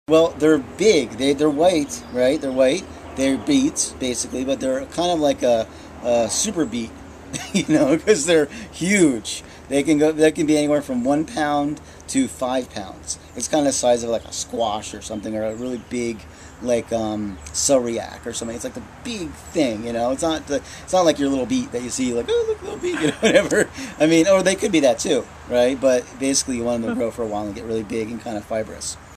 What is a sugar beet? - Michigan non GMO sugar (1)
Brian Steinberg explains his vision for a Michigan non GMO sugar beet industry.